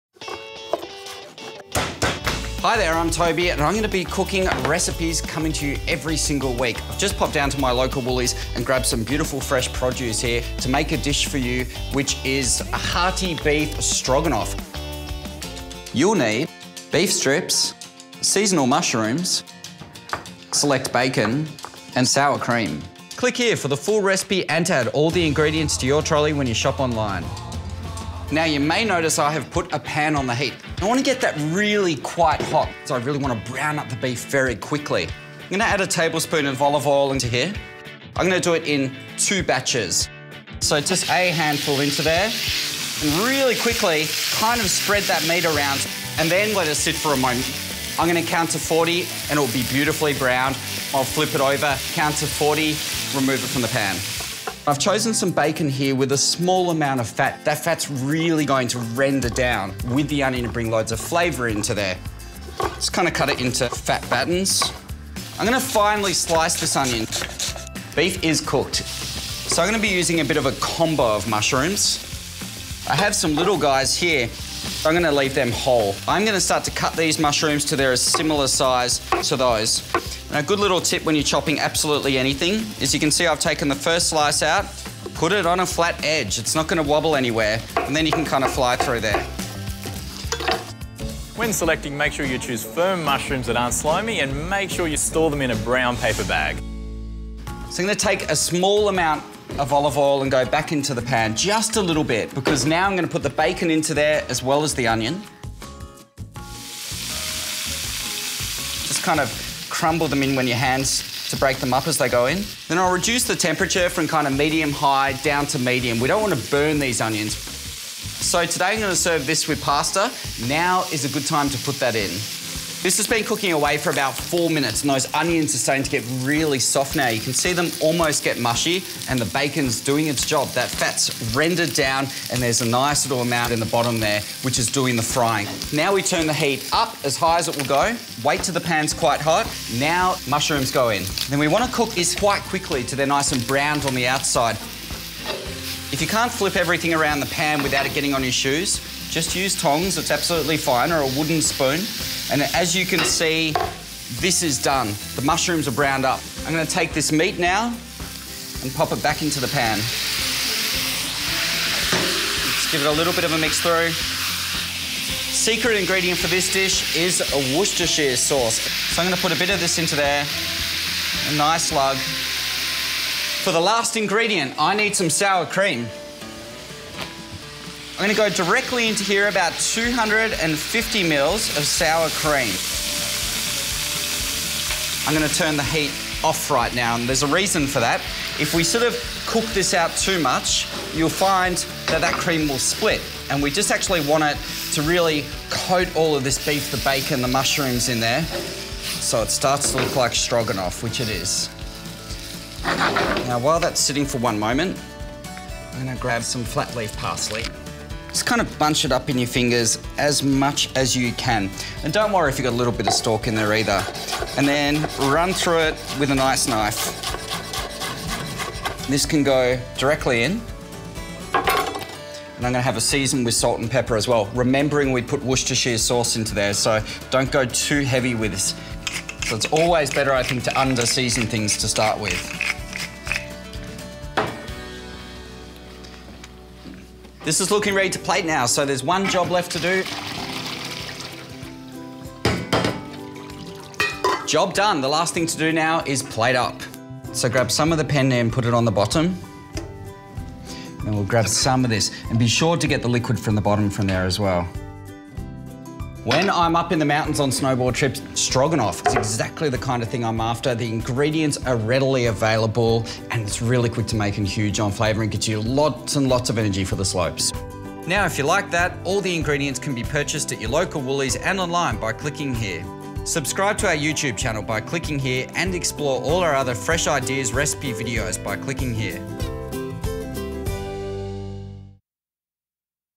Beef Stroganoff Recipe
A quick beef recipe- easy family dinner using beef stir fry, bacon, mushrooms, sour cream & pasta. Can serve with green beans.

Heat the pan, brown off the beef. Sauté onion and garlic in same pan. Add the mushrooms until tender. Re-turn beef to pan with sauce. Remove from heat, stir in sour cream and parsley. Serve over penne pasta.

Prep mins: 15
Cook mins: 25
Serves: 4

Ingredients:
2 tablespoons Woolworths Select Spanish classic olive oil
700g beef stir fry heart smart strips
175g packet Woolworths Select bacon short cut rindless, cut into thick batons
1 large red onion, thinly sliced
350g mixed mushrooms, sliced
2 tablespoons Woolworths Select Worcestershire sauce
250g Woolworths sour cream
1 cup fresh continental parsley leaves, coarsely chopped
375g Woolworths Select dried penne pasta

Method:
1. Heat 1 tablespoon of oil in a large frying pan over medium-high heat. Cook beef, in two batches, until browned. Transfer to a plate.
2.  Add remaining oil, bacon and onion to same pan. Cook, stirring for 5 minutes or until onion is soft. Add mushrooms. Cook stirring, until tender. Return beef to pan with Worcestershire sauce. Bring to boil, simmer 1 minute. Remove from heat, stir in sour cream and parsley until combined. Season.
3. Meanwhile, cook pasta in a large saucepan of boiling salted water until tender, drain.
4. Serve stroganoff over pasta.

Tip 1: For a richer flavour try adding 1 teaspoon Woolworths Select Dijon mustard and ½ teaspoon Woolworths Select ground paprika.
Tip 2: You can also use sliced rump steak in this recipe.
Tip 3: Beef stroganoff is traditionally served with pasta; however, try it with rice or mashed potatoes. Try serving it with steamed green beans, sautéed broccoli or a crisp salad.

Shop all the ingredients from this recipe and more on Woolworths'